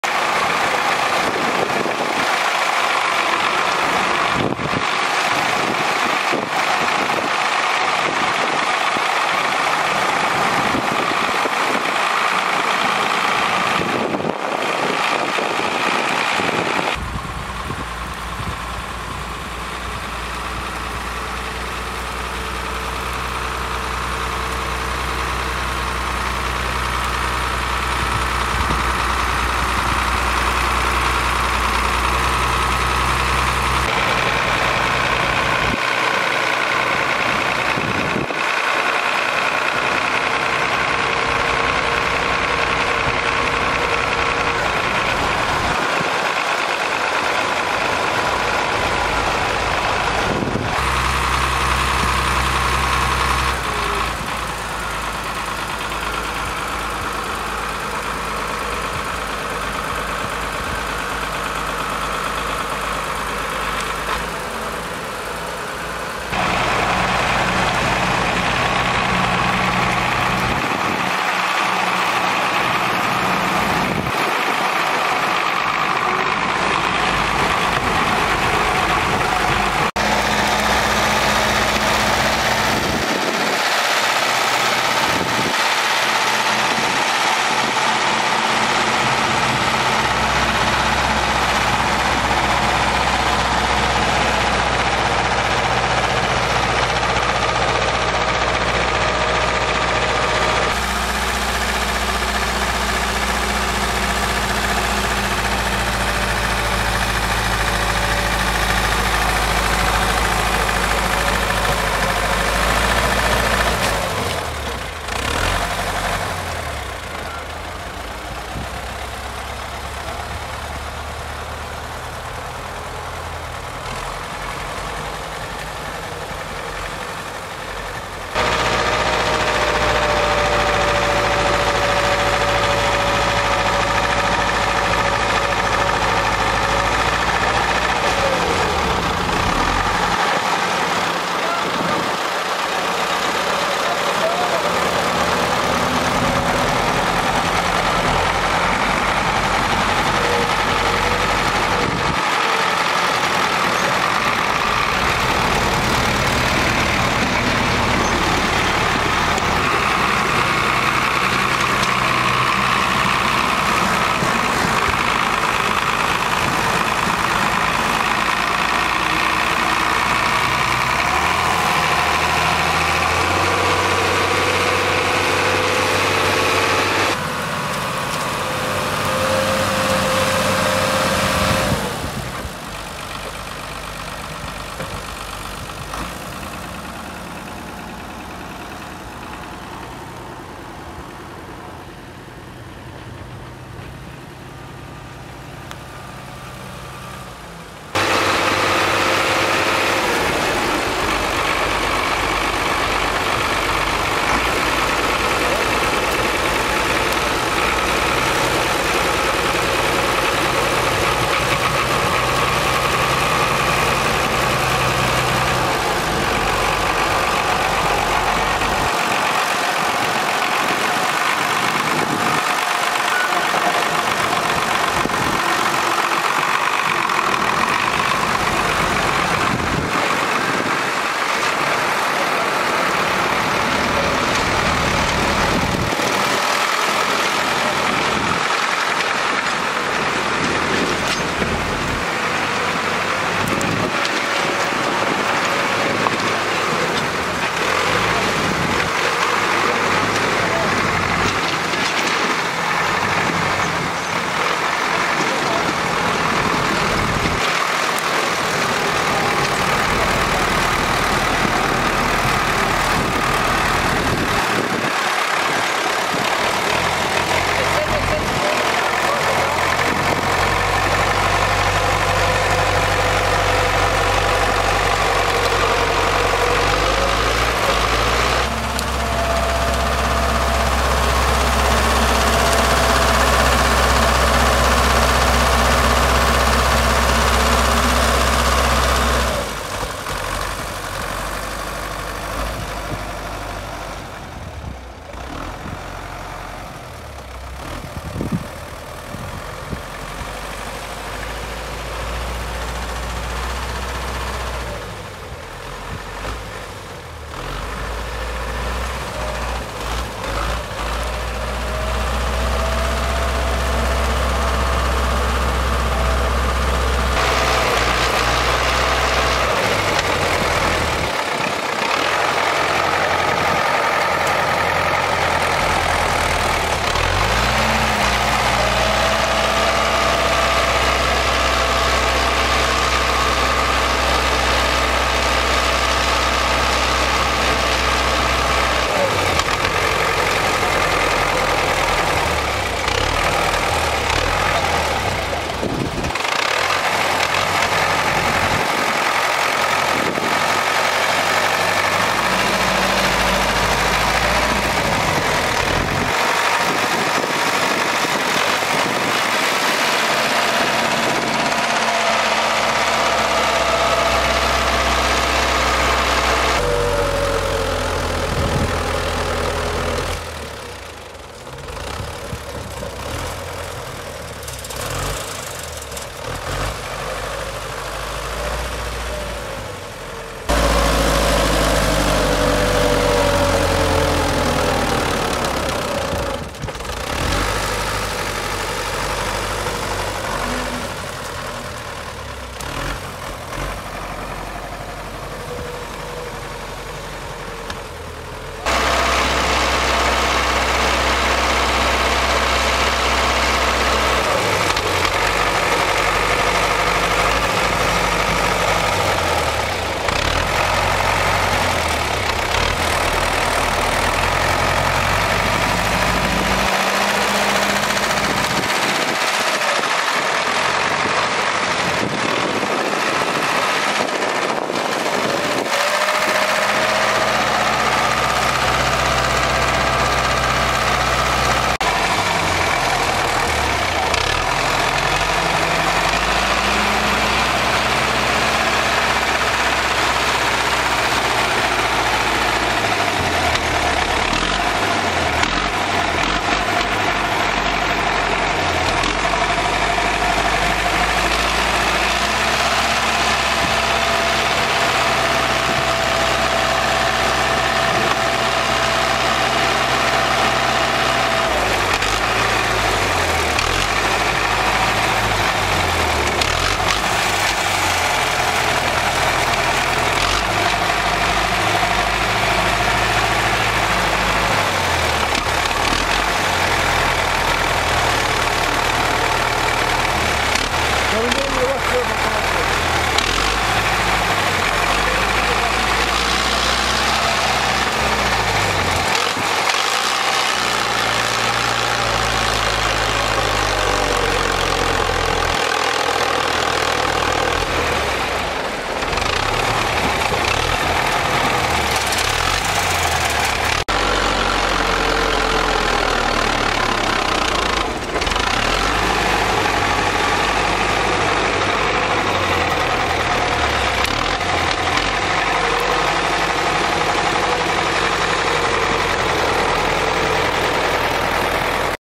Сравнили Тракторов на Подъем | Трактор Беларус 12221 против Трактор Беларус 82
✅ Зароботайте на бинарный опционах ✅ Счёт ЮМОНЕЙ Для Заказа Видео Роликов Сравнили Тракторов на Подъем | Трактор Беларус 12221 против Трактор Беларус 82